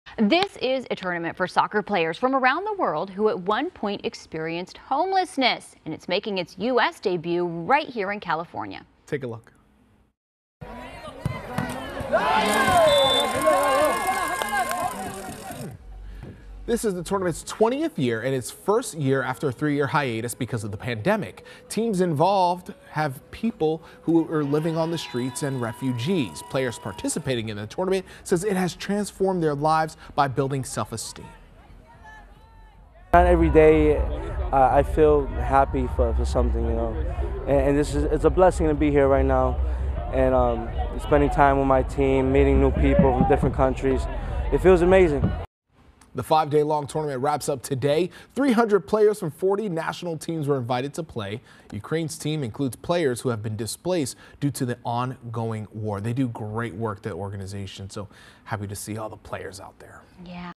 Homeless World Cup makes United States debut in California
The five-day tournament, which comprises of teams from around the world whose players were once homeless, took place at California State University, Sacramento and featured 300 players from 40 different national teams.